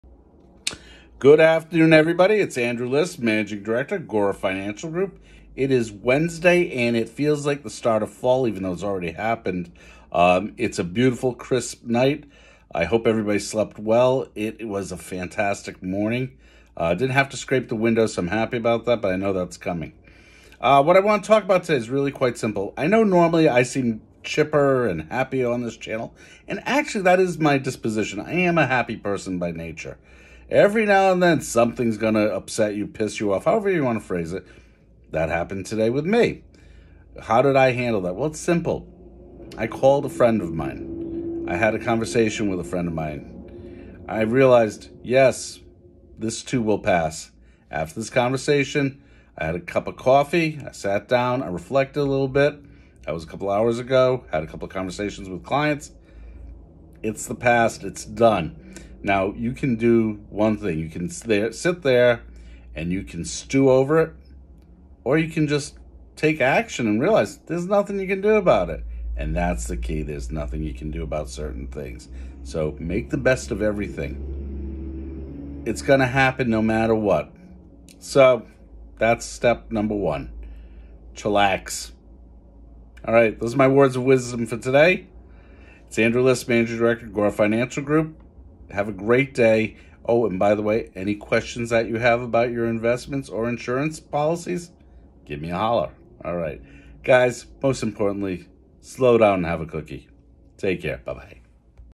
Don’t let some aggravating news destroy your day.
It always happens you’re having a great day, and then something￼ screws the day up. This happened to me today, call someone, have a cup of coffee and let it pass. It is not worth a heart attack or a stroke the faster on this stuff. Have a great day.